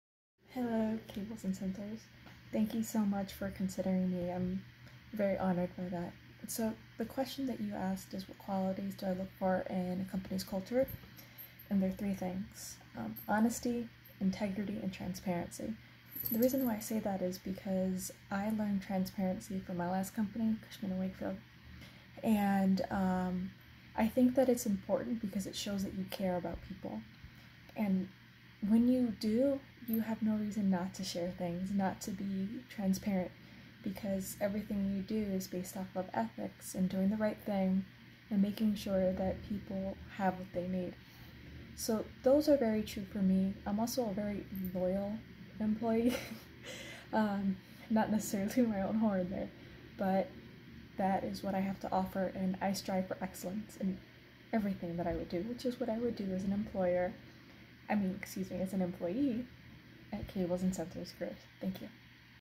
Video Interview for Cables & Sensors (company) _ Vlog 198
This is an interview I did for Cables & Sensors Per their request, I added it on YouTube. I'm grateful they want to be a part of such a wonderful group, the youtube audience. My audience values inclusion and belonging. Thank you!

Time Stamps
0:00 Intro
0:05 What I look for in a companies culture

Feel free to say hi! :-)

Created by InShot Video Editor